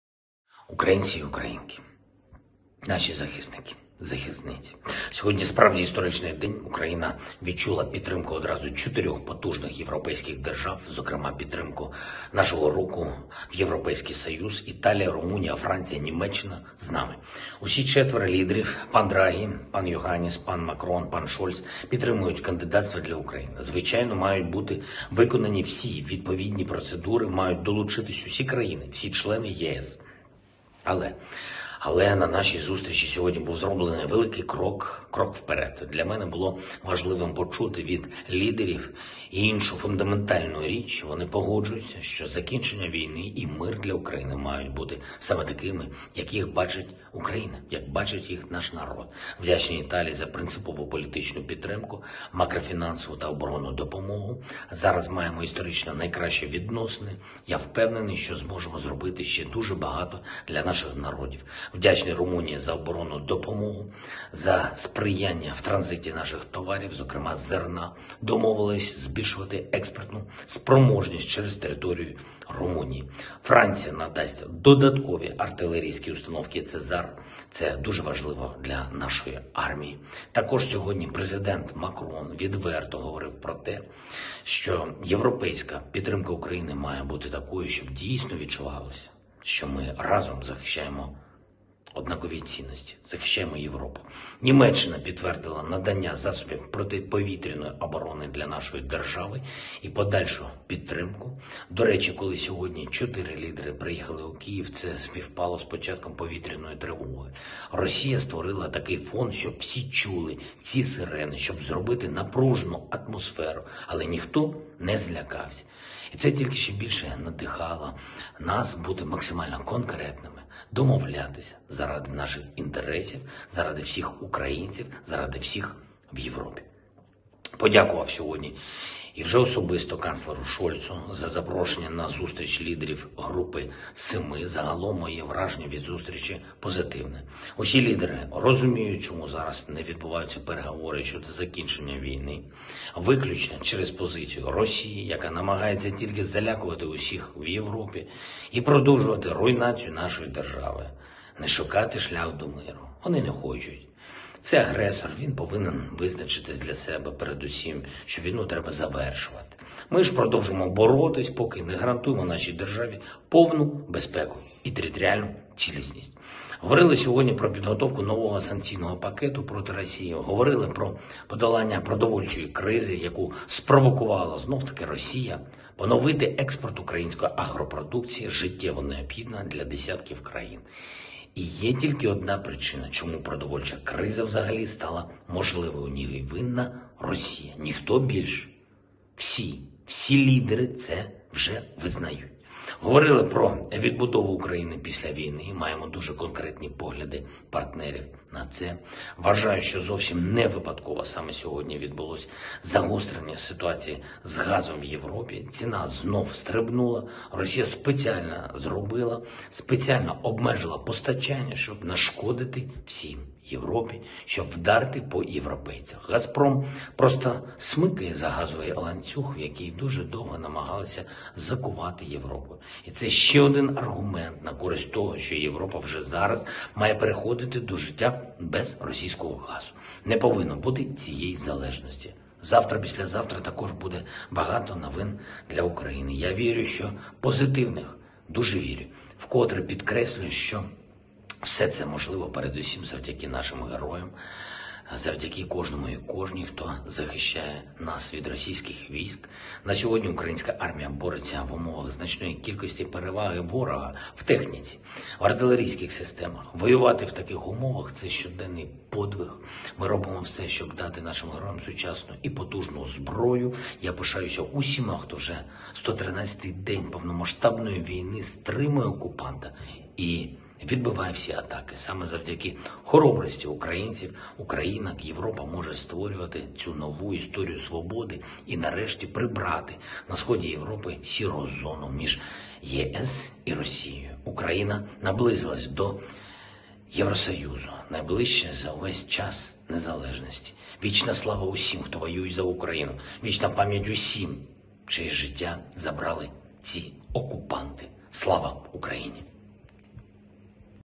Zelenskyy appeal 2022.06.16
Today is a truly historic day - Ukraine has felt the support of four powerful European states at once. And in particular support for our movement to the European Union. All leaders understand why negotiations to end the war are not under way. Exclusively because of Russia's position, which is only trying to intimidate everyone in Europe and continue the destruction of our state. They do not want to look for a way to peace. We will continue to fight until we guarantee our state full security and territorial integrity.